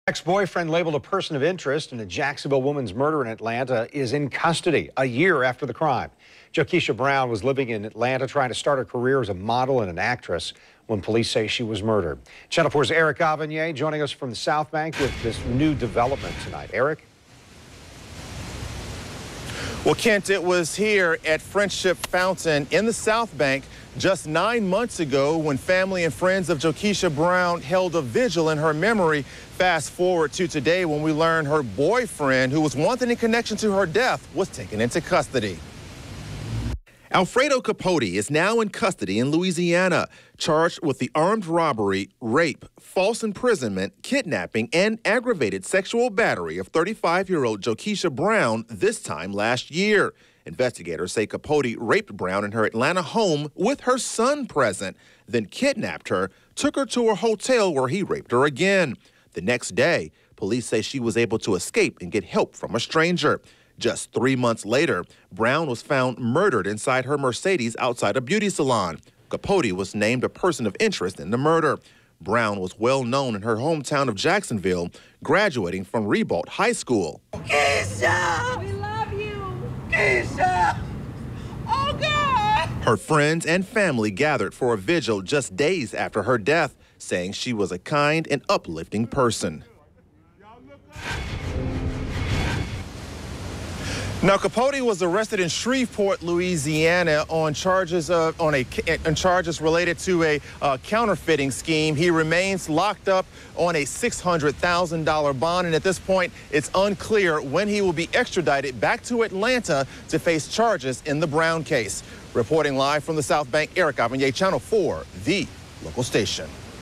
‘Person of interest' in Jacksonville native's death arrested in Louisiana
VIDEO: The ex-boyfriend labeled a "person of interest" in a Jacksonville woman's murder in Atlanta is in custody a year after the crime.